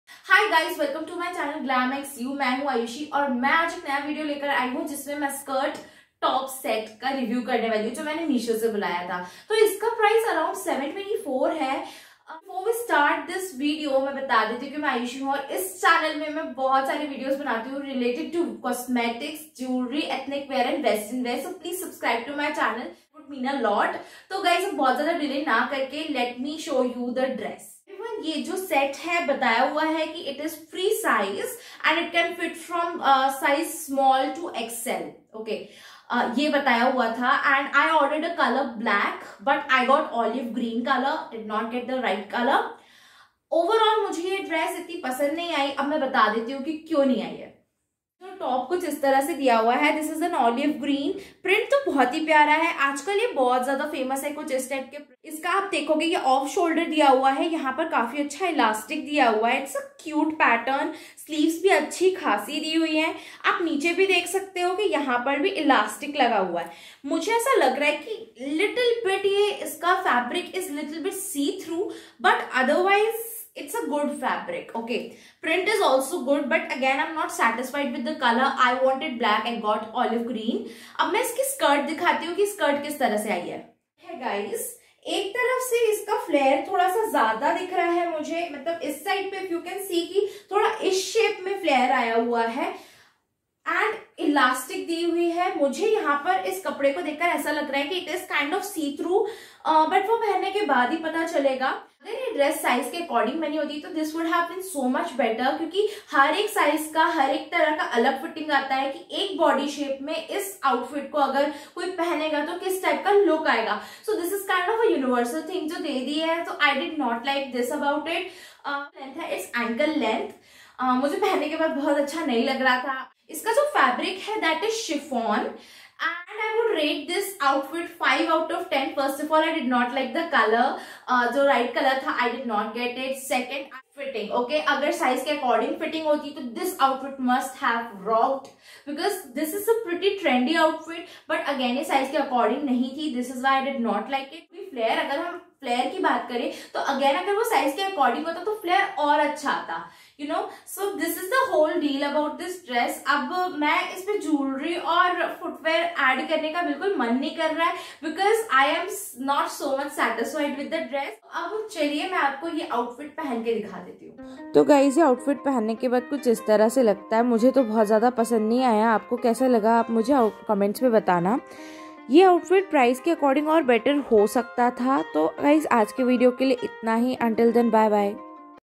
Meesho Dress Review Online India Haul Video| Skirt Top collection for Girls| Under 1000
Hello Guys!

In this video, I have reviewed a skirt top Set from Meesho. The set have some flaws. It is around 800  I hope you liked this video.

This video also covers the following topics:

Meesho Haul, meesho dress review in hindi,
meesho fashion